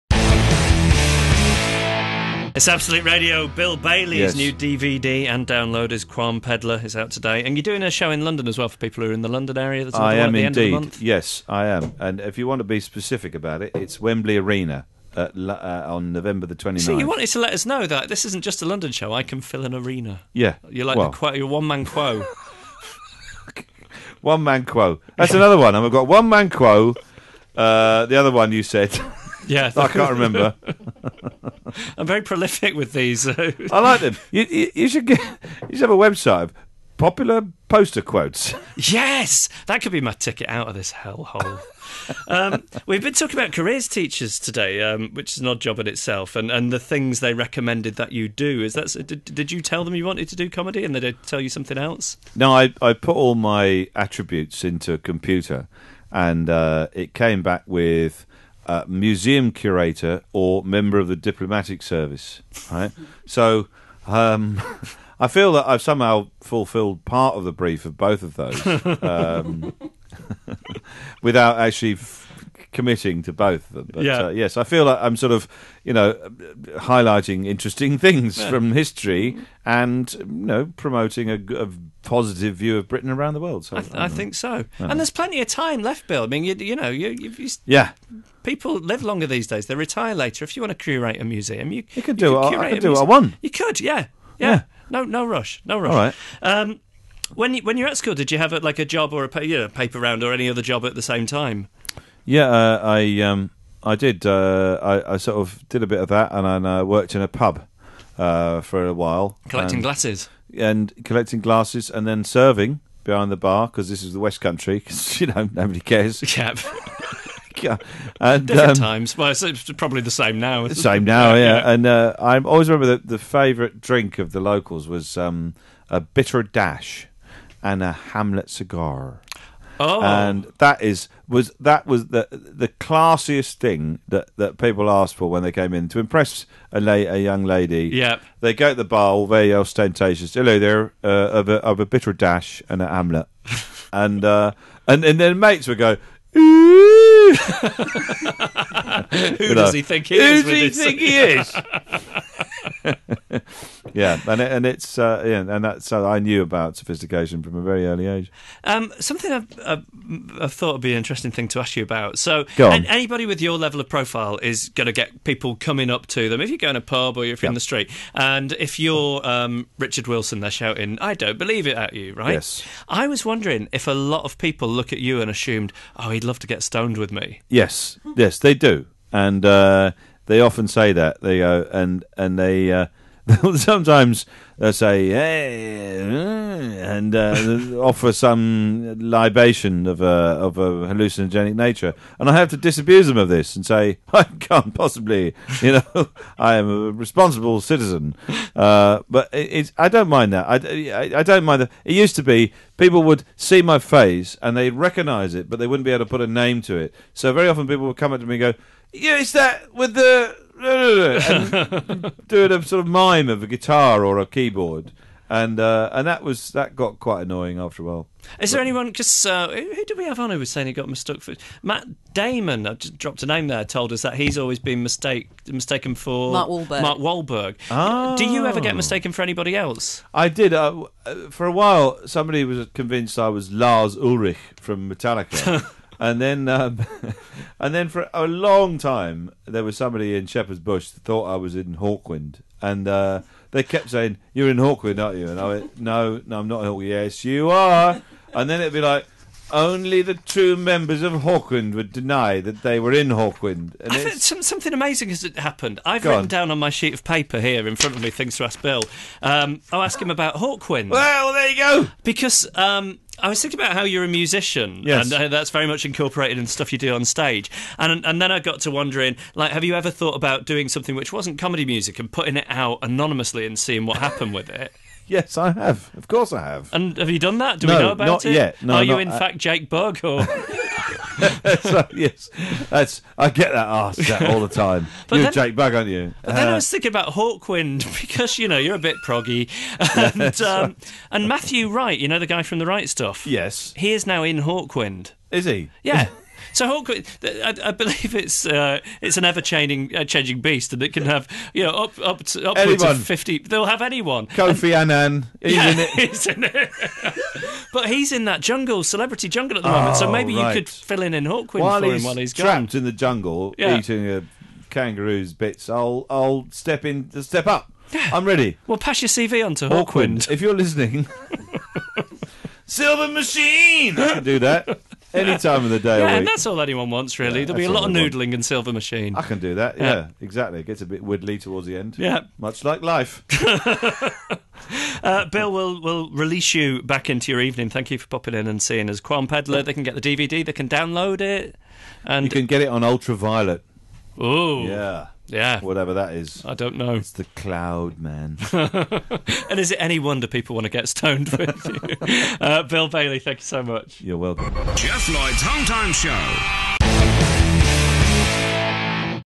Bill Bailey 'Qualmpeddler' interview on Absolute Radio (part 2 of 2)
In part 2 of Geoff Lloyd's interview with Bill Bailey, topics under discussion include career choices, and getting mistaken for rockstars - as well as Bill's new show 'Qualmpeddler'.